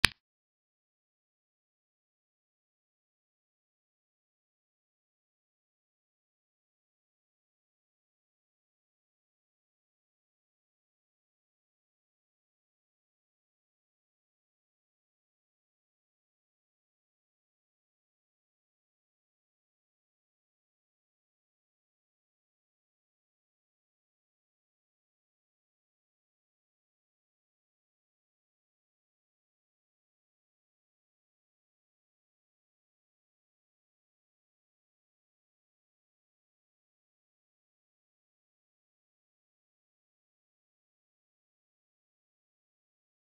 Zelda Compass of Light Animation of 2013
Flipnote views2 Downloads0 Total Stars Comments0 RegionEurope and Oceania Flipnote ID50419 ChannelSudomemo

I had this based off an animation called "The Legend of Zelda: Compass of Light" which was made by iZelda27 some years ago.